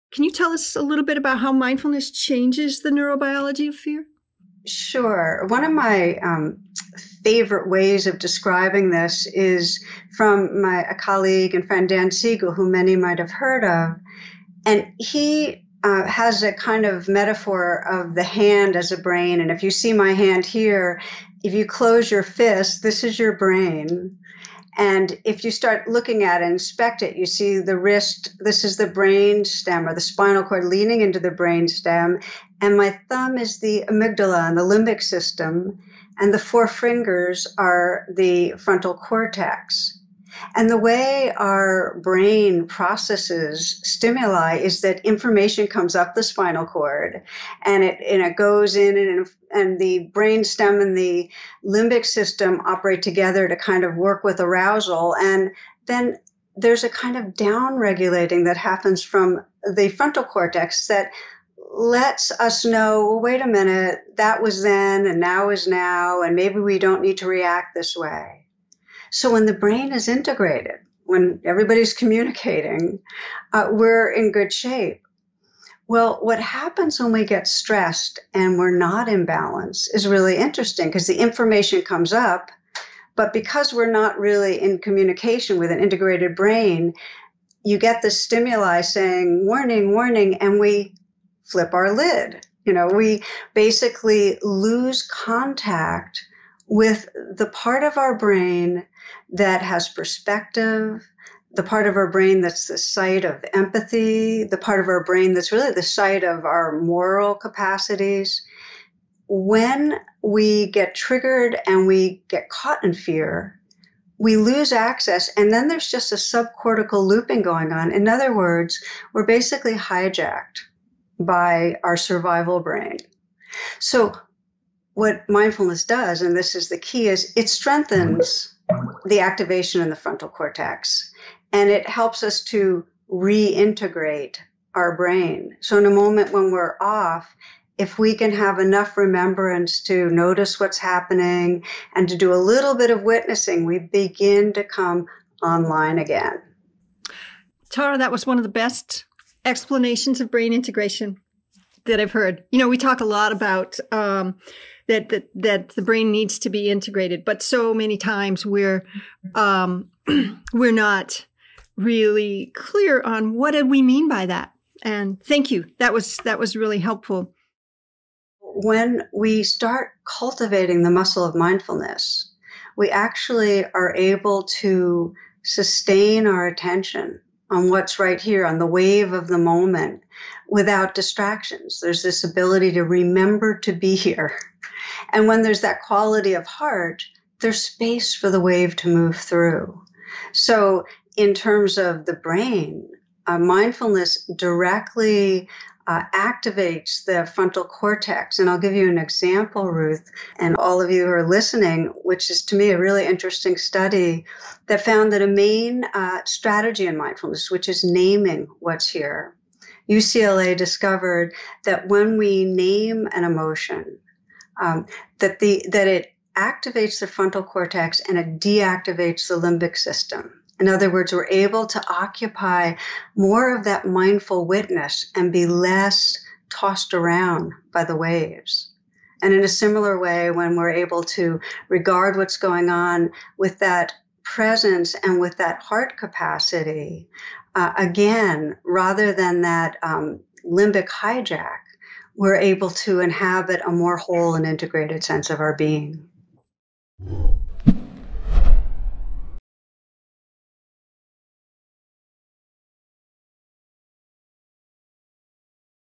The Neuroscience of Mindfulness and Fear with Tara Brach, PhD
When a client is afraid, there’s a practice that can help them stay grounded in the present . . .

. . . instead of getting hijacked by panic and “worst-case scenario” thinking.

And that practice is mindfulness.

In this video, Tara Brach, PhD will get into how mindfulness disrupts the neurobiology of fear, and she does it with a powerful metaphor that you could share with your clients.

This teaching comes from the NICABM blog.